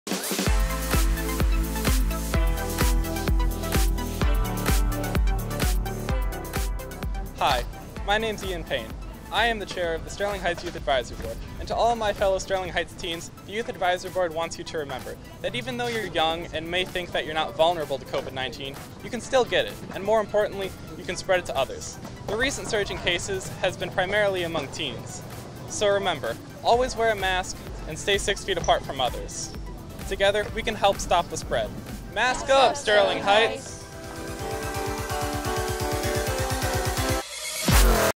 Mask Up, Sterling Heights: A Message from the Y.A.B.
The Sterling Heights Youth Advisory Board is asking area teens to help us beat COVID-19 by Masking Up.